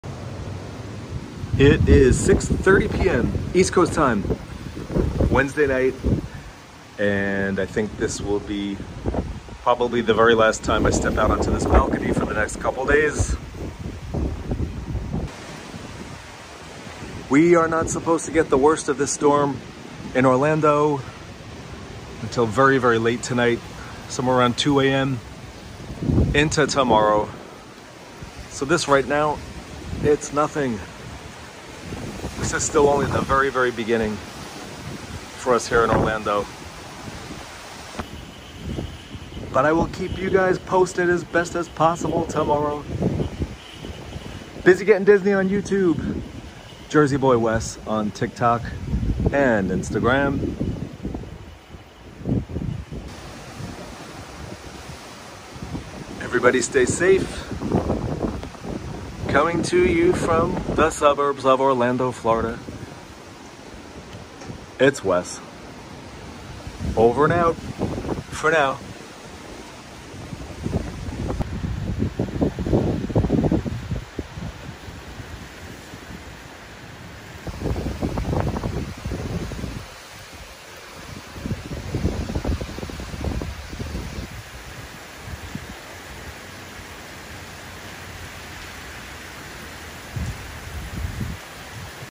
Hurricane Ian Coming In Hot!
One quick YouTube check-in before closing all doors and windows for the night! Everyone stay safe!